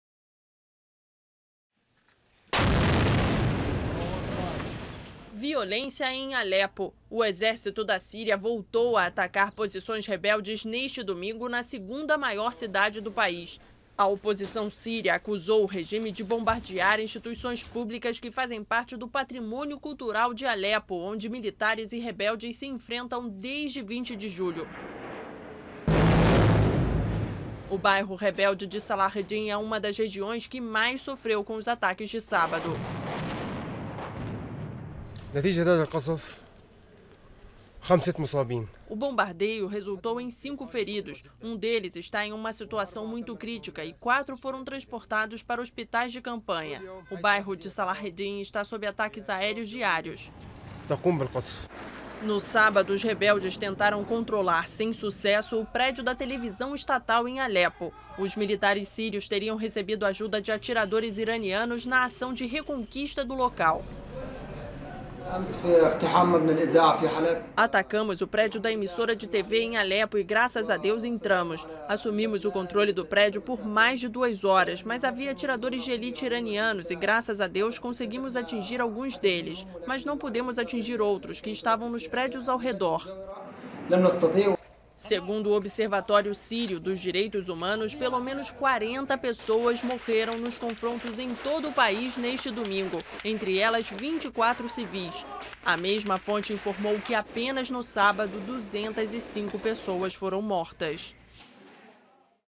Exército sírio ataca Aleppo
O exército da Síria voltou a atacar posições rebeldes no domingo em Aleppo, segunda maior cidade do país. O Observatório Sírio dos Direitos Humanos informou que pelos menos 13 pessoas morreram durante os combates. No sábado, segundo os rebeldes, atiradores iranianos teriam ajudado os militares sírios a recuperarem o controle do prédio da televisão estatal em Aleppo.